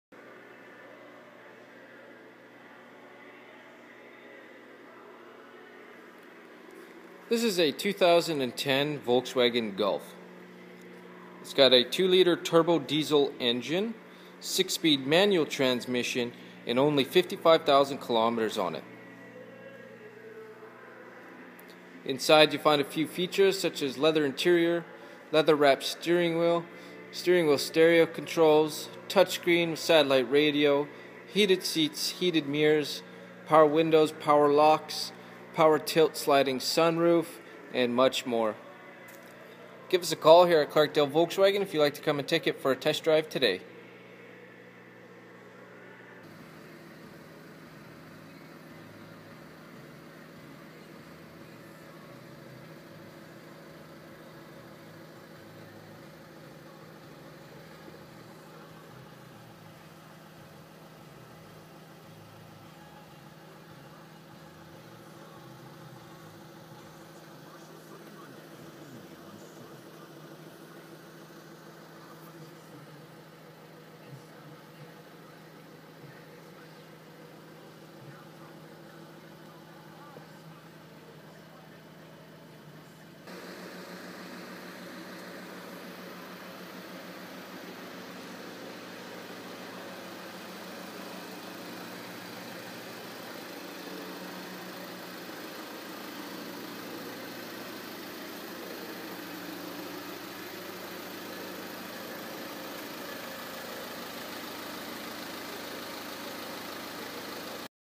2010 Volkswagen Golf 4dr HB Man TDI | Clarkdale Volkswagen
This is a 2010 Volkswagen Golf 4dr HB Man TDI with 6-Speed M/T transmission White[B4B4,Candy White] color and Titan Black interior color.  This video is recorded and uploaded by cDemo Mobile Inspector